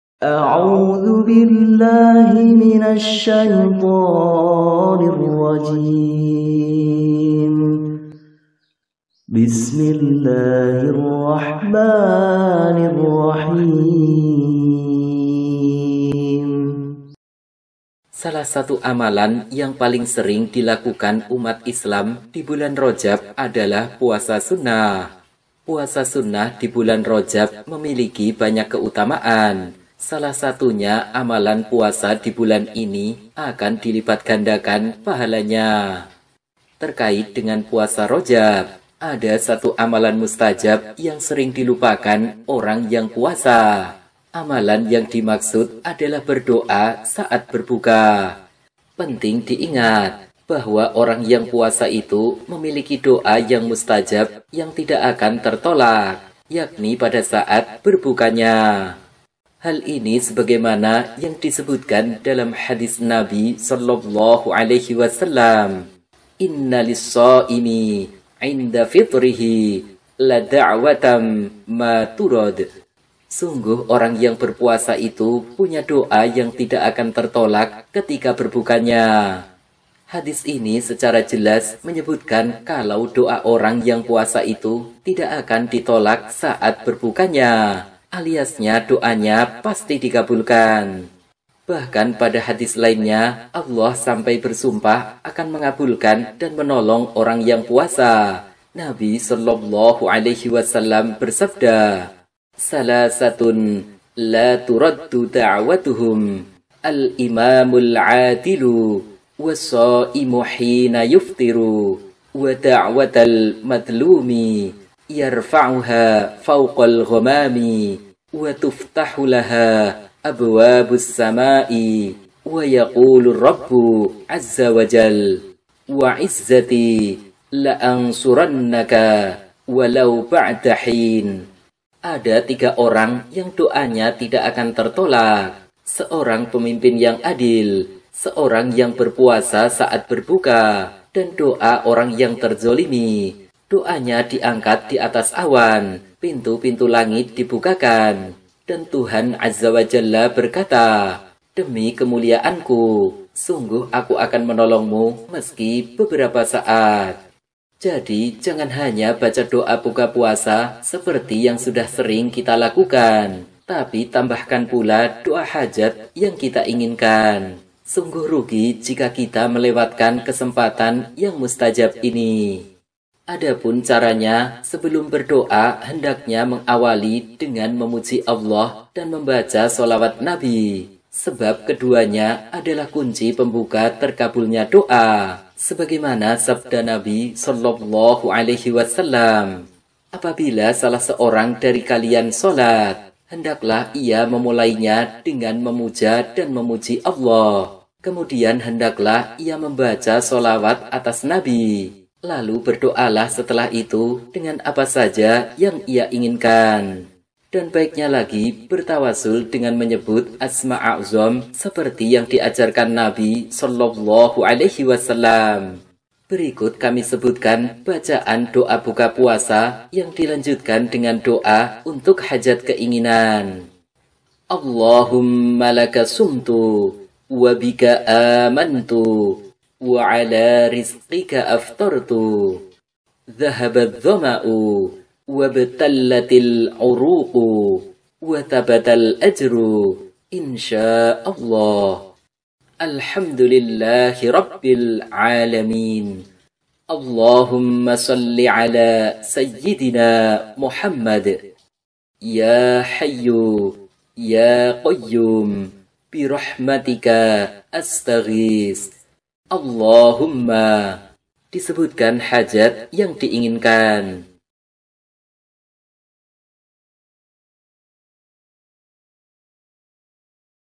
Puasa Rajab, Jangan Lupa Awal Maghrib Lakukan Amalan Ini - Ust. Mahmud Asy-Syafrowi
Bagi Yang Puasa Rajab, Awal Maghrib Jangan Lupa Lakukan Amalan Ini

Salah satu amalan yang paling sering dilakukan umat Islam di bulan Rajab adalah puasa sunnah.
Ada satu amalan mustajab yang sering dilupakan orang puasa saat sedang berbuka.
Penting diingat, bahwa orang yang puasa itu memiliki doa yang
mustajab yang tidak akan tertolak, yakni pada saat berbukanya.
Jadi, jangan hanya baca doa buka puasa saja, seperti yang sudah sering kita lakukan. Tapi tambahkan pula doa hanjat yang kita inginkan. Sungguh rugi jika kita melewatkan kesempatan yang mustajab ini.
Berikut ini kami sebutkan bacaan doa buka puasa, yang dilengkapi
dengan doa untuk memohon hajat keinginan.
Semoga bisa memberi manfaat untuk kita semuanya, aamiin yaa robbal 'aalamiin.

Mahmud Asy-Syafrowi